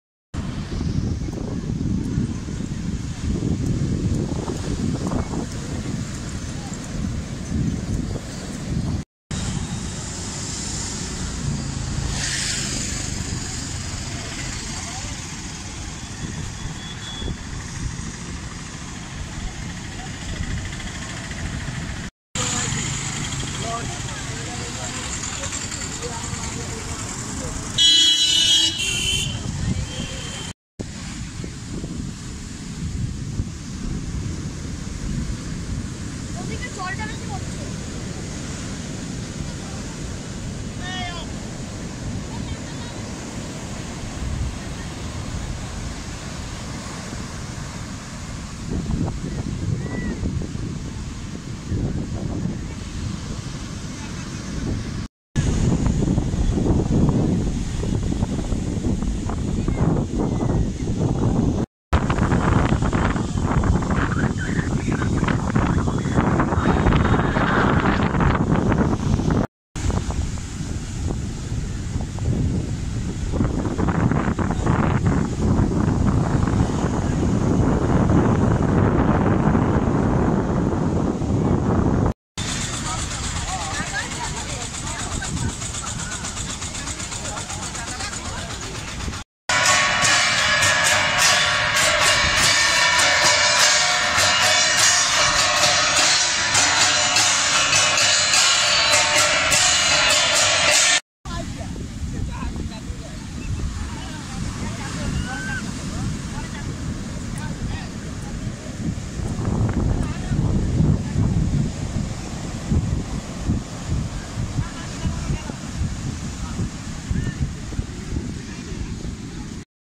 Puri Tour
A Tourist group has arrived at Puri Sea Beach in the morning and waiting to get a hotel of their desire. Hotel brokers are now very busy to catch them for their contract hotels. This has been described in this video.

Ashoke Dey